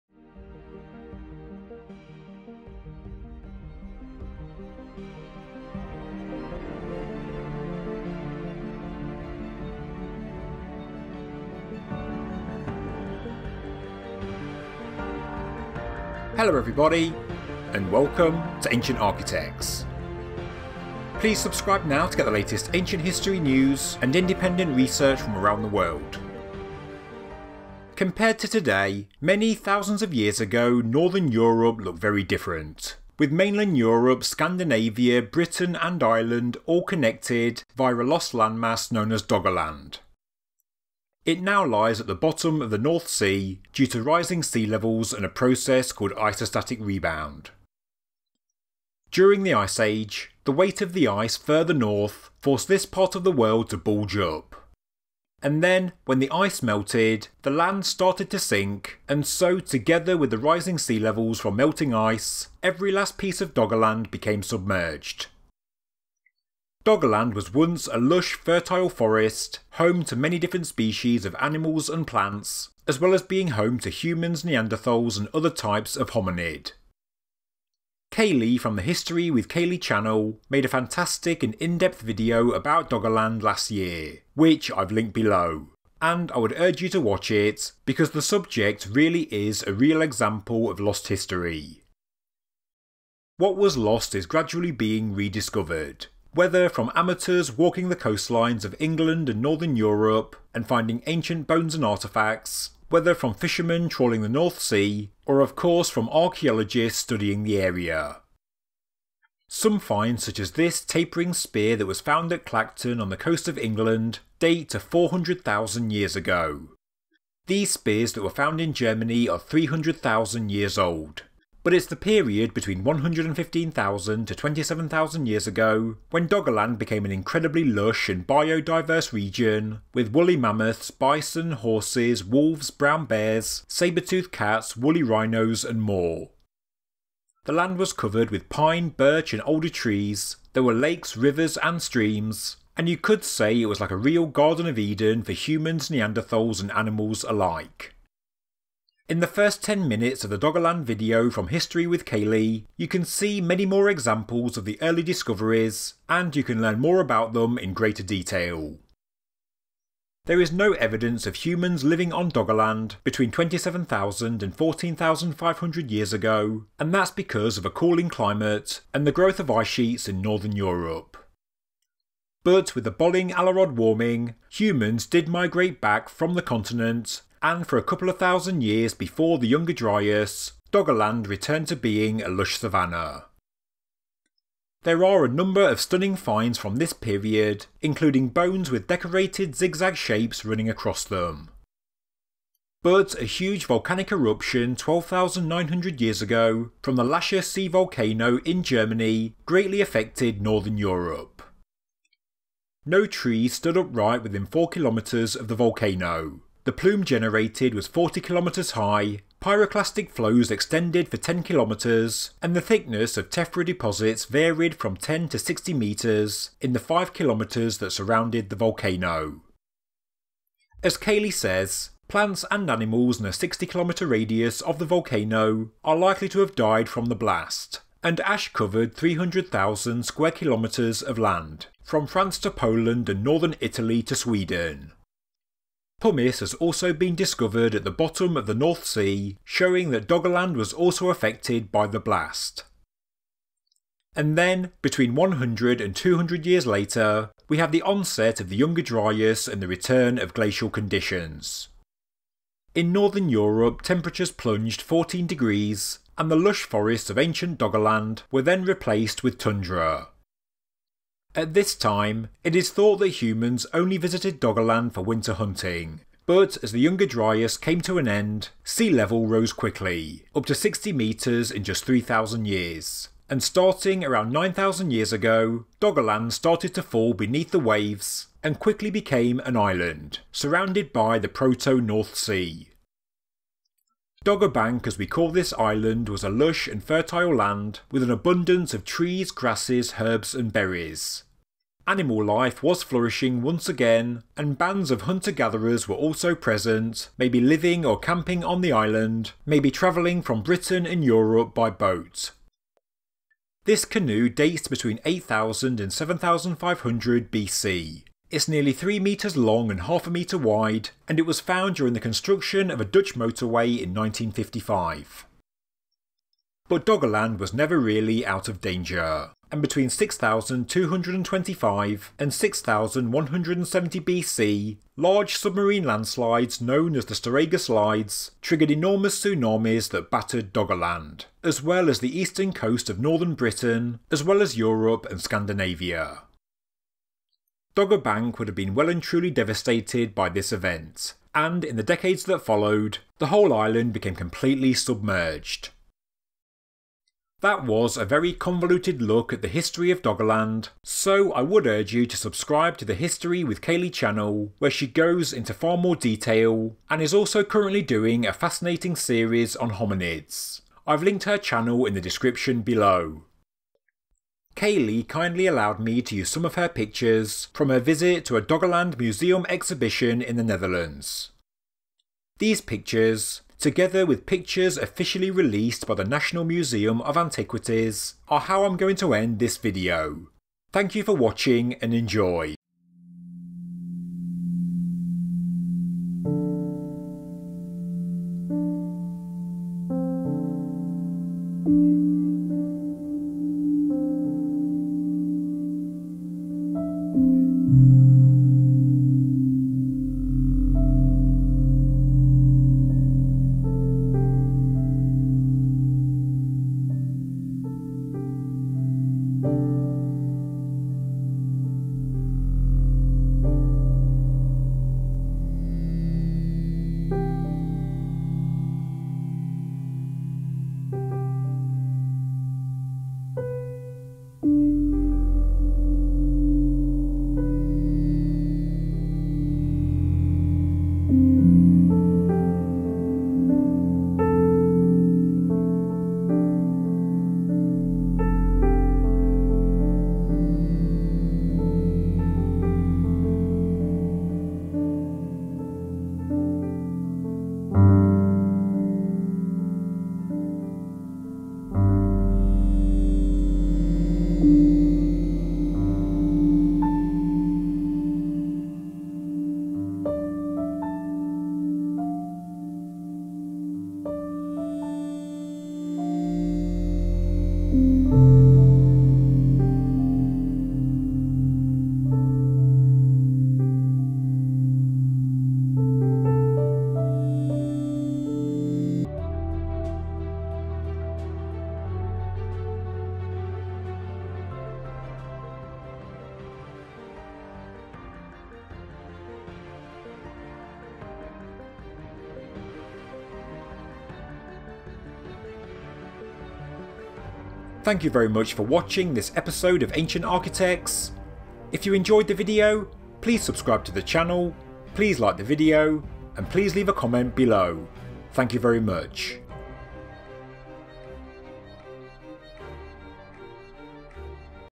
Doggerland Discoveries: The Incredible Lost and Found Artefacts | Ancient Architects
Many thousands of years ago Northern Europe looked very different compared to today, with Britain and Ireland connected to mainland Europe and Scandinavia, via a lost landmass known as Doggerland.

It now lies at the bottom of the North Sea due to rising sea levels and a process called Isostatic rebound. During the ice age, the weight of the ice further north forced this part of the world to bulge up. When the ice melted, the land started to sink until every last piece of Doggerland became submerged.

Doggerland was once a lush forest, home to many different species of animals and plants, as well as being home to humans, Neanderthals and other types of hominid.

Kayleigh, from the History with Kayleigh channel, made a fantastic and in-depth video about Doggerland in August 2021  and I would urge everyone to watch it because this subject really is an example of lost history.

But what was lost is gradually being rediscovered, whether from amateurs walking the Eastern Coast of England and the northern coast of Europe and Scandinavia, and finding ancient bones and artefacts, whether from fisherman trawling the North Sea and pulling up ancient harpoons and mammoth skulls, or of course from archaeologists studying the area.

In this video I give you a history of the lost landmass of Doggerland and at the end of this video, I'll show you some of the incredible lost and found artefacts that were recently on display at the Rijksmuseum van Oudheden in the Netherlands - I hope you enjoy it!